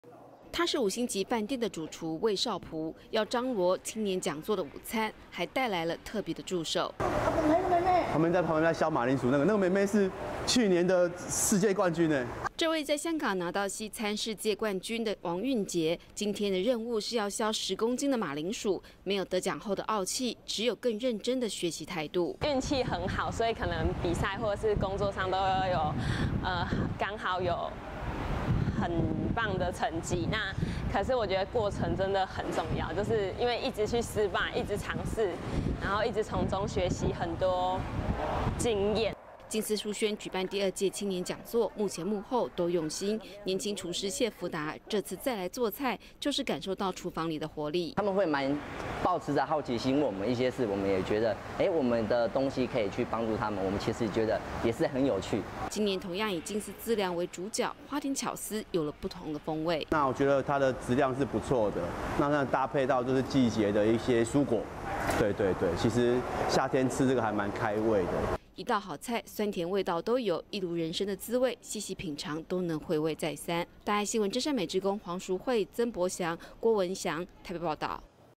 靜思書軒青年講座 身心靈饗宴
周末二日，靜思書軒舉辦青年講座，五星級的飯店主廚魏韶璞，與徒弟王韻捷，用淨斯資糧做出餐點，讓人驚嘆，處處看見年輕人對工作的熱忱。

他是五星級飯店的主廚，魏韶璞，要張羅青年講座的午餐，還帶來了特別的助手。

主廚 魏韶璞：「(哪一個妹妹) 旁邊在削馬鈴薯，那個妹妹是去年的世界冠軍。」

這位在香港拿到西餐世界冠軍的王韻捷，今天的任務是要削十公斤的馬鈴薯，沒有得獎後的傲氣，只有更認真學習的態度。

廚師 王韻捷：「運氣很好，所以可能比賽或工作上，剛好有很棒的成績，可是我覺得過程真的很重要，因為一直失敗一直嘗試，一直從中學習很多經驗。」

靜思書軒舉辦第二屆青年講座，幕前幕後都用心，年輕廚師謝福達，這次再來做菜，就是感受廚房裡的活力。

廚師 謝福達：「他們會滿抱持著好奇心，問我們一些事，我們也覺得我們的東西可以幫助他們，我們也覺得是很有趣。」

今年同樣以淨斯資糧為主角，花點巧思，有不同的風味。

主廚 魏韶璞：「我覺得它的質量是不錯，搭配到季節的一些蔬果，夏天吃這個還滿開胃。」

一道好菜，酸甜味道都有，一如人生的滋味，細細品嘗，都能回味再三。

真善美志工 黃淑惠 曾柏翔 郭文祥 台北報導